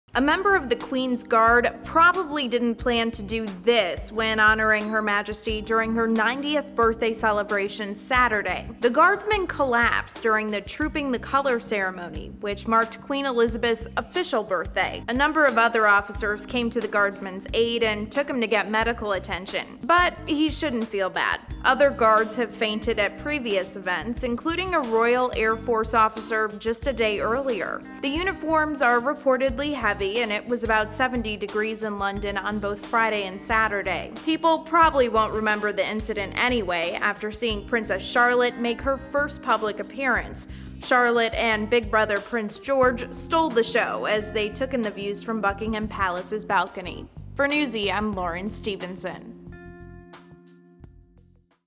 Man Down! Member Of Queen's Guard Faints During Birthday Ceremony
A member of the Queen's Guard probably didn't plan to do this when honoring Her Majesty during her 90th birthday celebration Saturday.

The guardsman collapsed during the Trooping the Colour ceremony, which marked Queen Elizabeth's "official" birthday.

A number of other officers came to the guardsman's aid and took him to get medical attention.

But he shouldn't feel bad. Other guards have fainted at previous events, including a Royal Air Force officer just a day earlier. The uniforms are reportedly heavy, and it was about 70 degrees in London on both Friday and Saturday.

People probably won't remember the incident, anyway, after seeing Princess Charlotte make her first public appearance. Charlotte and big brother Prince George stole the show as they took in the views from Buckingham Palace's balcony.

This video includes images from Getty Images.